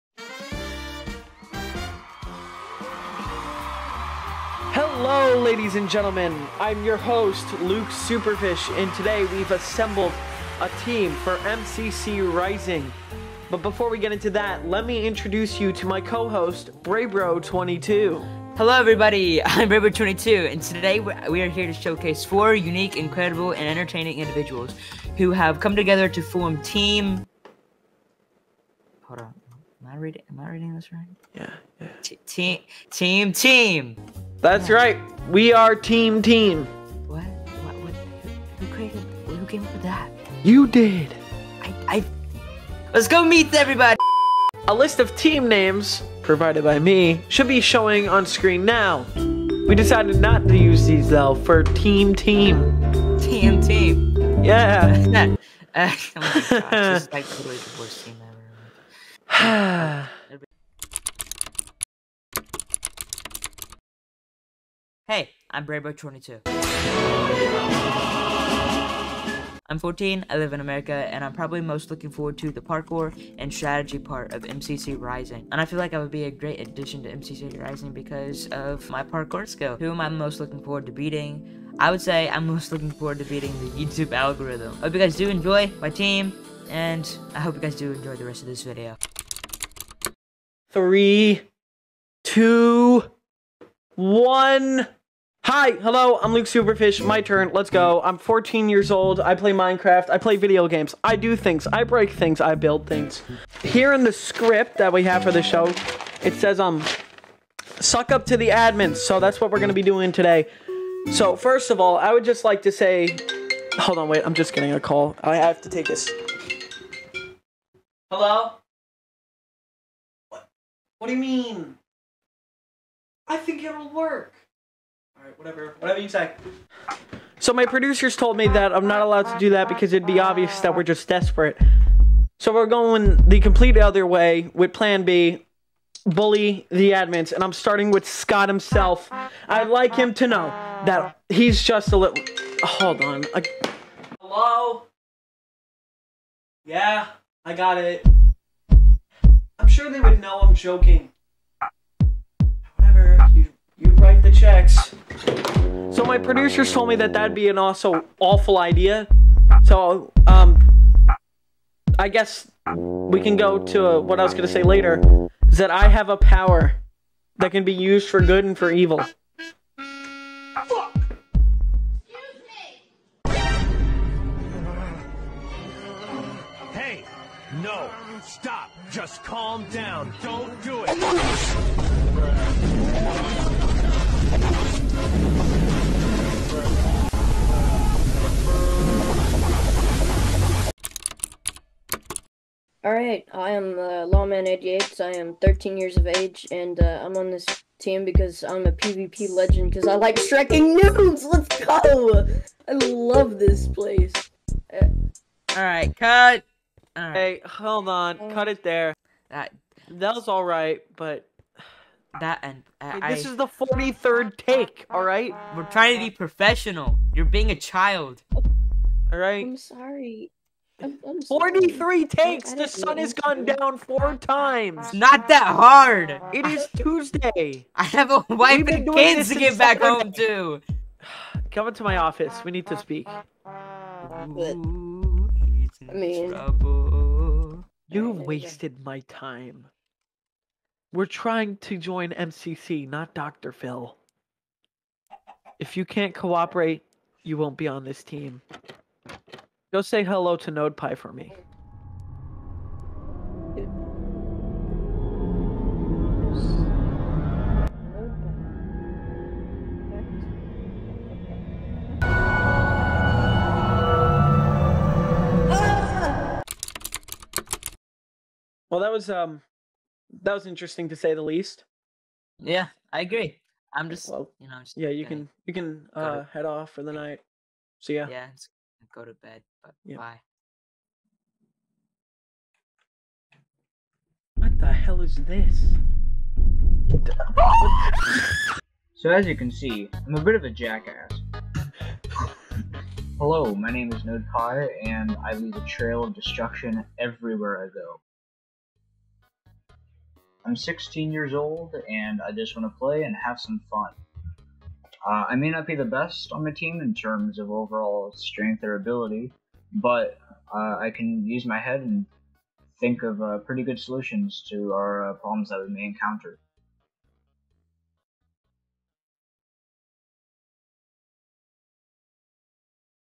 MCC-Rising Application | Team Team
Hello. Today, we will start (and hopefully not end) our journey to MCC. Team Team and I will hopefully be victorious, and I hope to see you in the event!